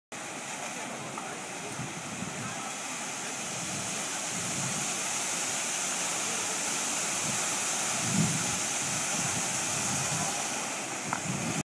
Huge Fountain In Nice, France.
The fountain at the end of the tramway in Nice.